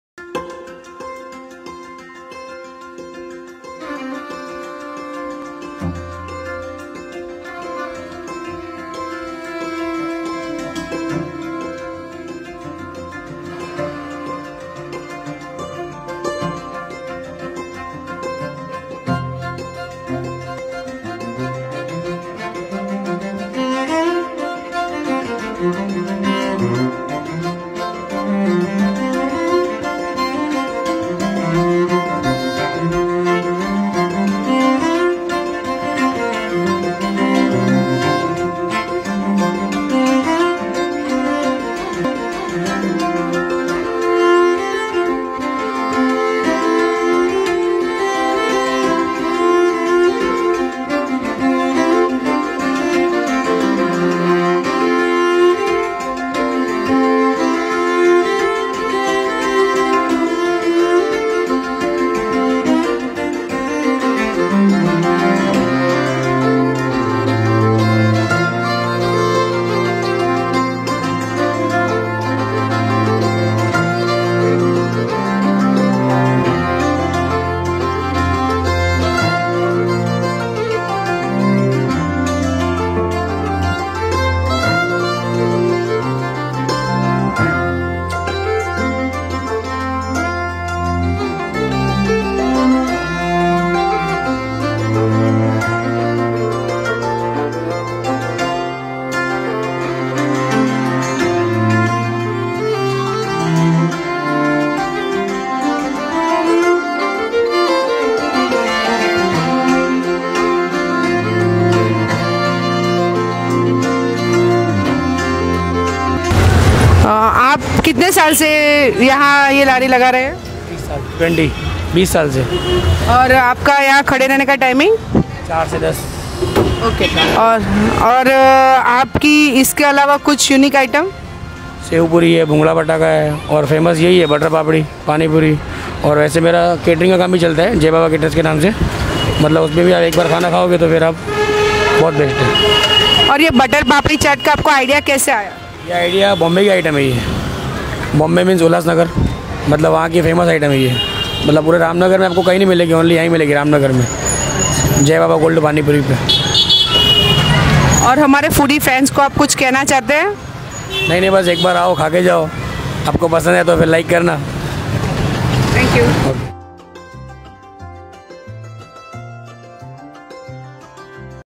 SURATI STREETS - BUTTER PAPADI
Surati Streets

In Today's Surat's food trip we will witness BUTTER PAPADI from Jay Baba Cold Panipuri - Nr. Neej Dhaam Ashram, Ramnagar, Rander, Surat

Jay Baba Cold Panipuri - Nr. Neej Dhaam Ashram, Ramnagar, Rander, Surat